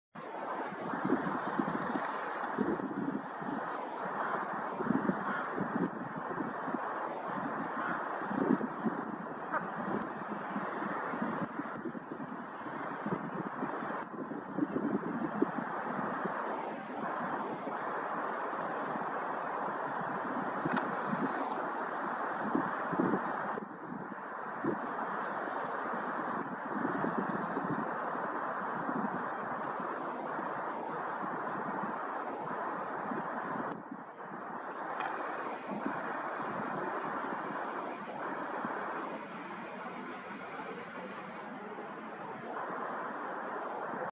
Hooded crow playing with branch: 02-11-2021
Hooded crows (Corvus cornix) are known for intellegency and learning through all life. So if they have free time, they sometimes like to play with different objects, in case on video – with piece of branch.
Nioman river bank in Hrodna city, W Belarus